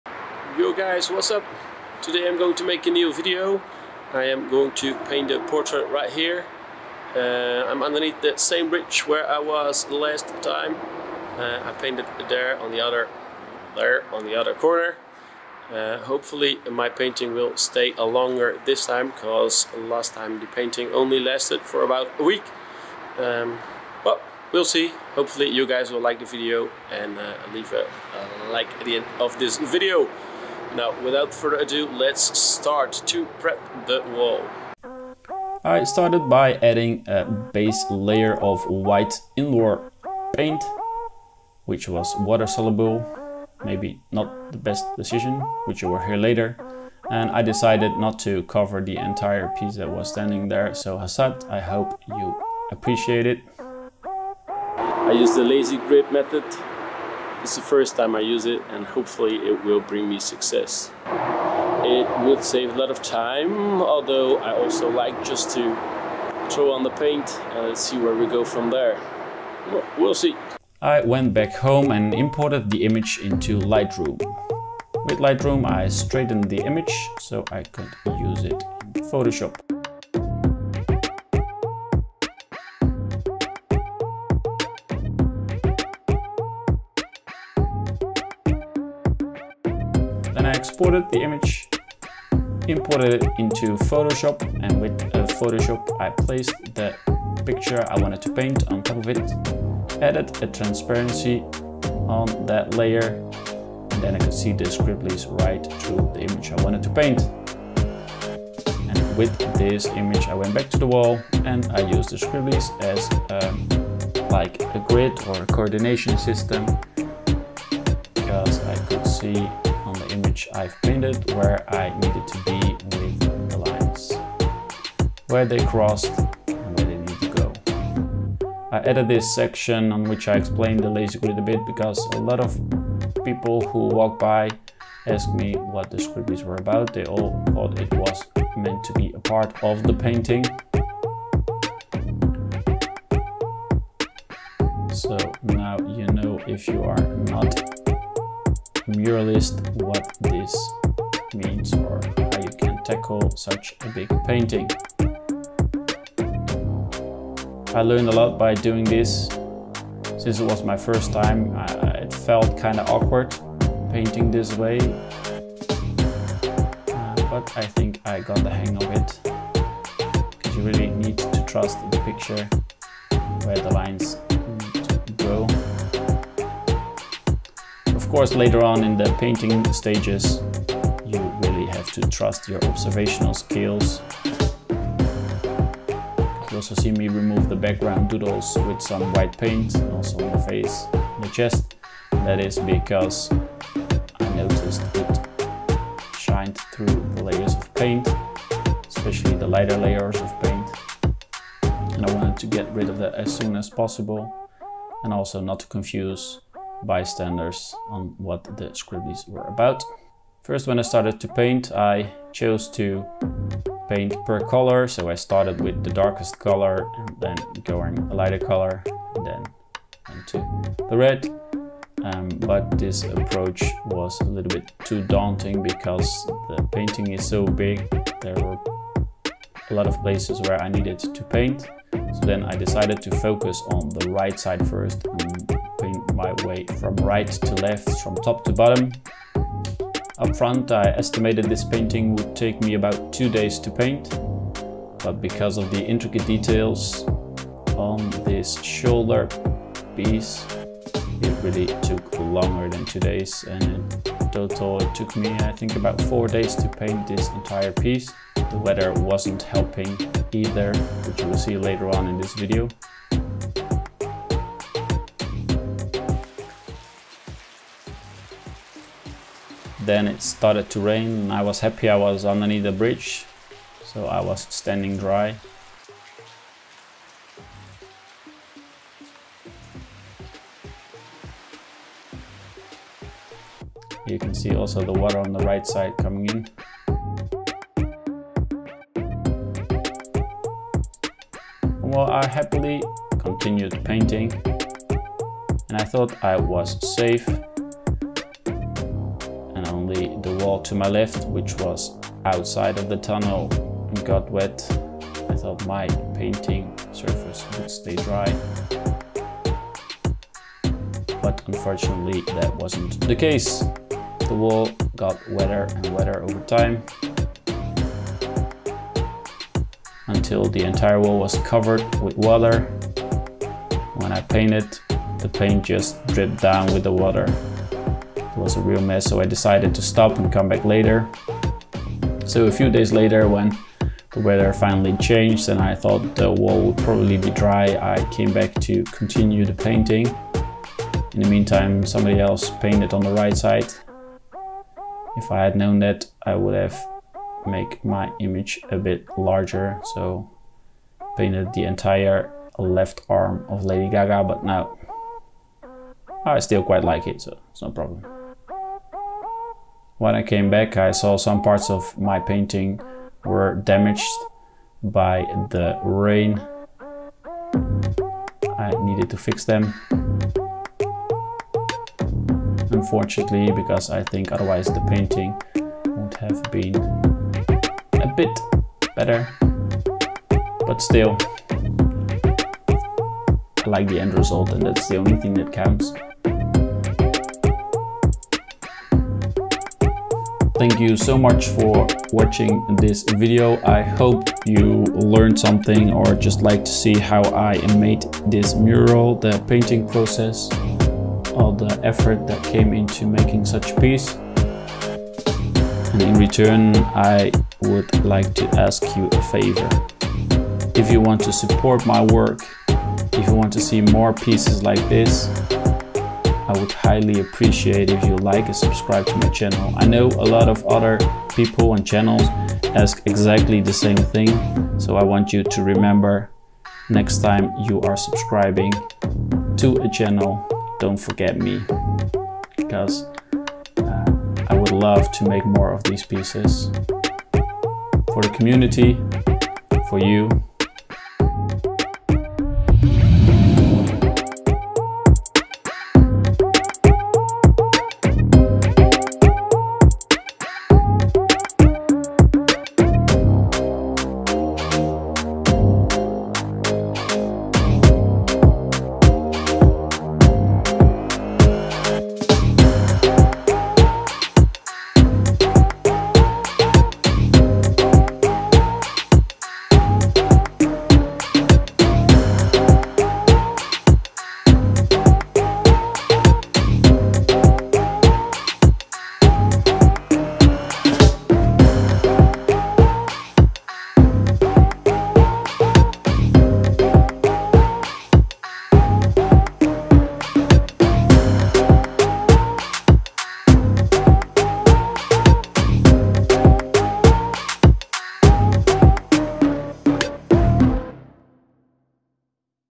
INSANE Graffiti Portrait Mural!!
In this video, I'll be showing you how I've painted a large graffiti portrait of Lady Gaga! The mural is made solely with spray paint. As a bonus I also explain how the lazy grid method works.